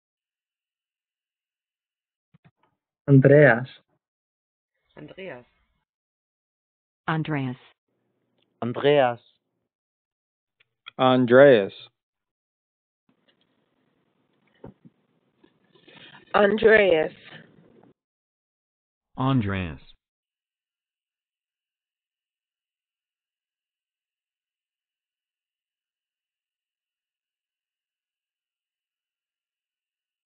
How to Pronounce Andreas
This video shows you how to pronounce Andreas. Record your own pronunciation, view the origin, meaning, and history of the name Andreas